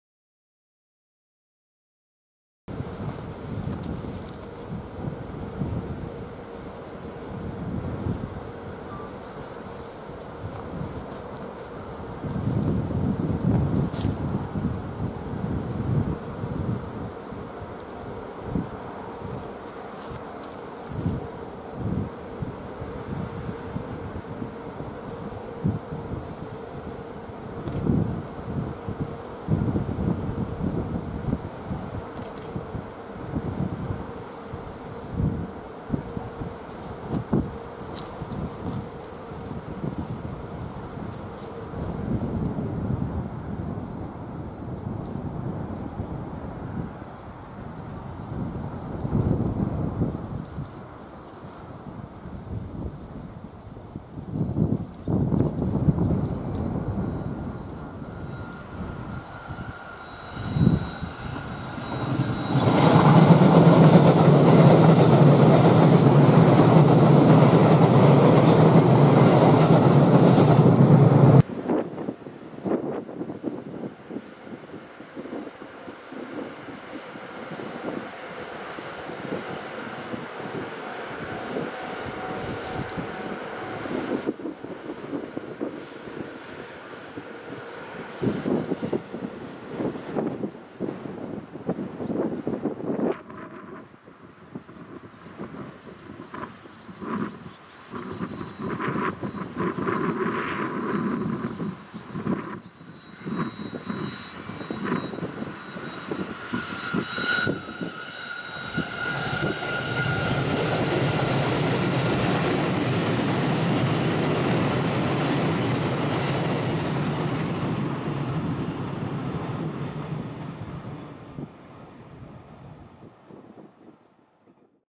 ２０７系体質改善車 普通 新三田行き （Z２+S38)
２０１７年１月４日
福知山線　三田駅～新三田駅間
オール更新車のため撮影しました。
列車番号　１１７７C
編成番号　Z2+S38
行き先　新三田
※強風のため風の音が入っていますがご了承ください。